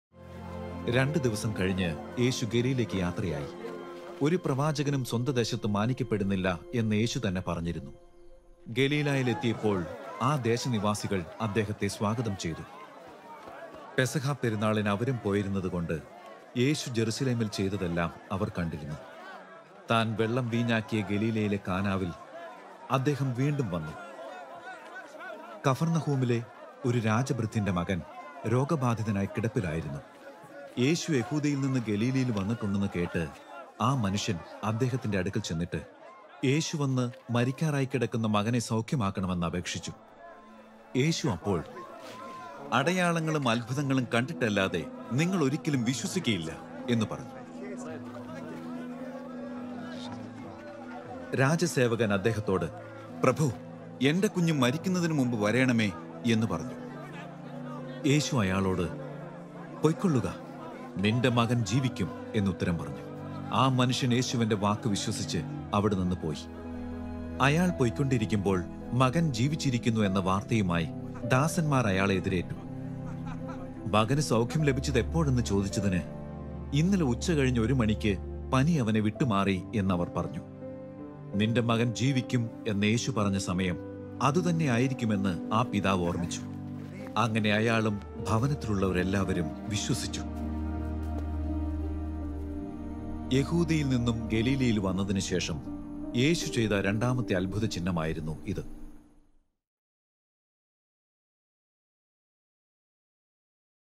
രാജഭൃത്യന്റെ പുത്രനെ സുഖപ്പെടുത്തുന്നു (Jesus Heals an Official’s Son)
John 4:43-54

മലയാളം ഗോസ്പൽ ഫിലിംസ് (Malayalam Gospel Films)